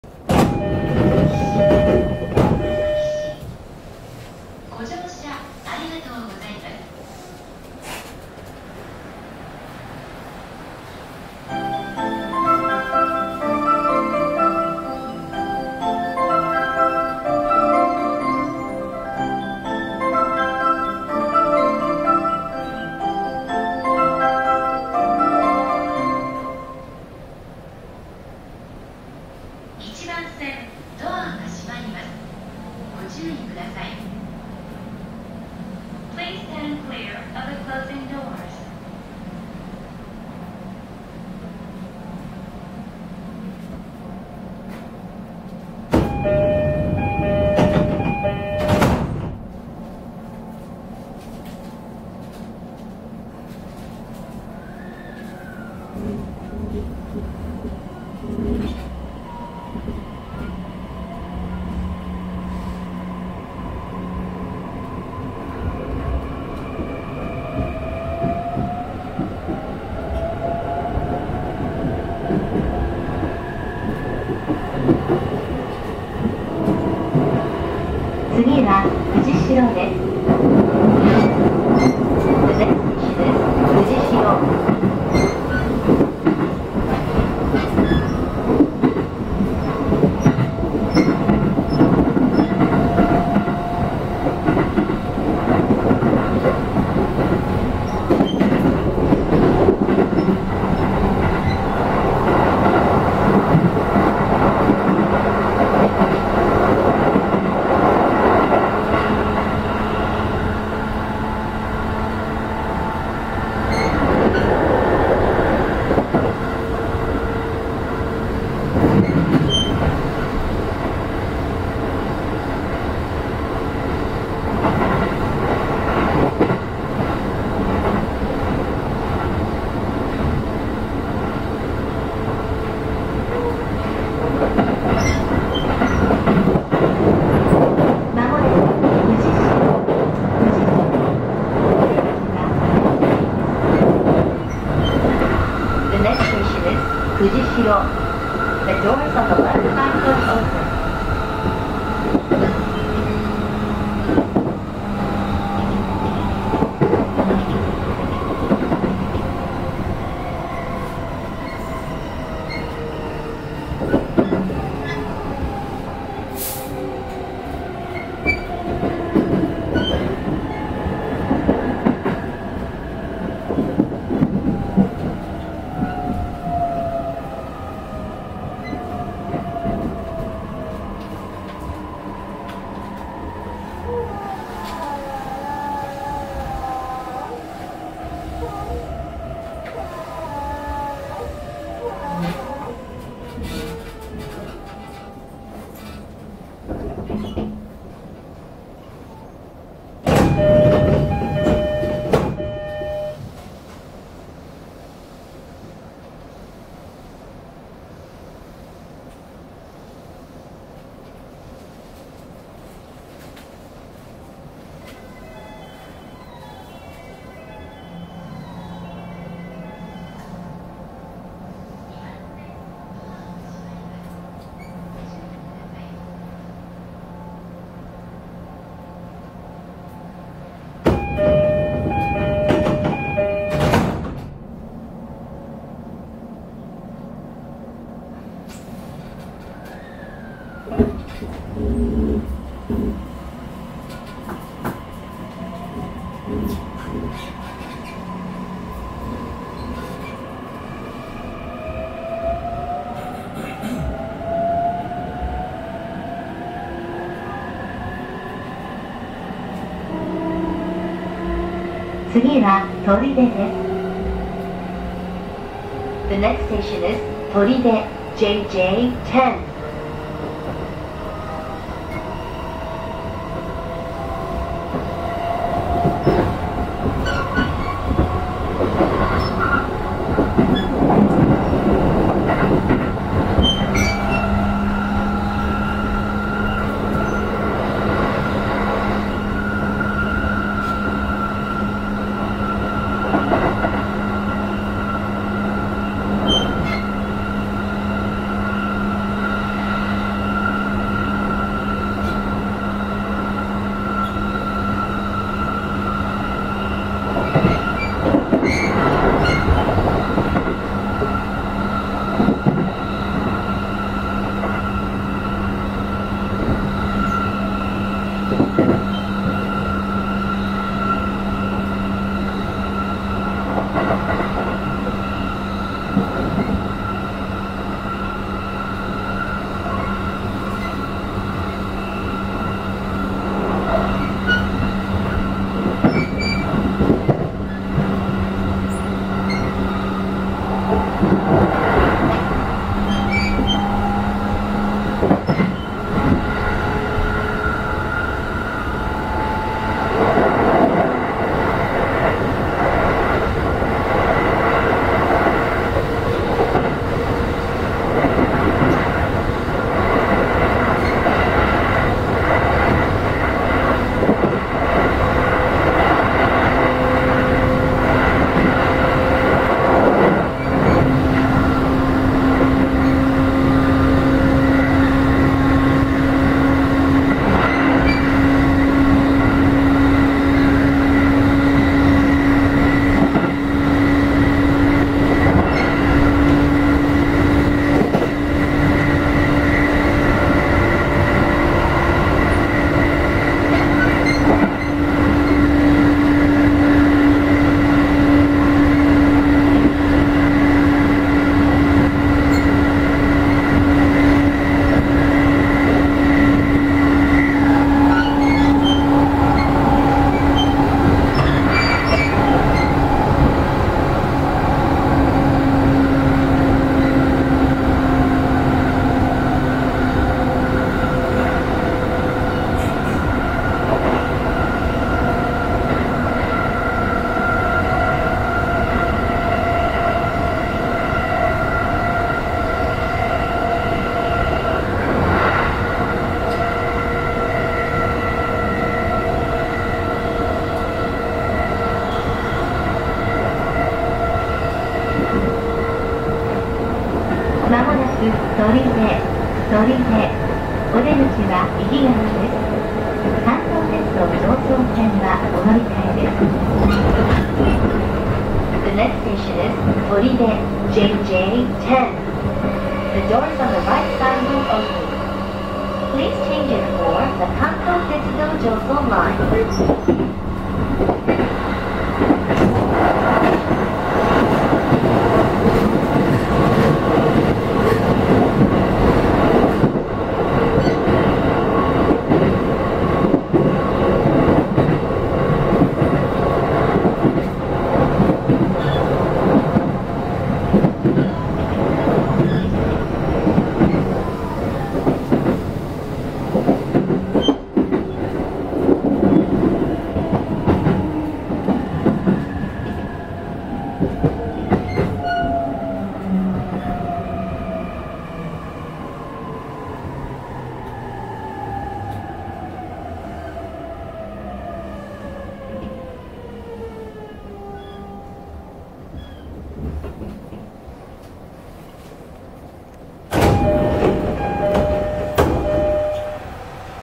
【走行音】常磐線 E531系 龍ケ崎市〜取手／Joban Line at the Ryugasakishi to Toride
常磐線　E531系　龍ケ崎市〜取手
龍ケ崎市駅の発車メロディがフルです

ものすごくわかりにくいですが、藤代駅発車後、
少しするとデッドセクションに入ります（04:45〜頃？）

◆発車シーンの目安
00:00　スタート
00:45　龍ケ崎市/Ryugasakishi
03:50　藤代/Fujishiro
07:26　取手（到着）/Toride(Arriving)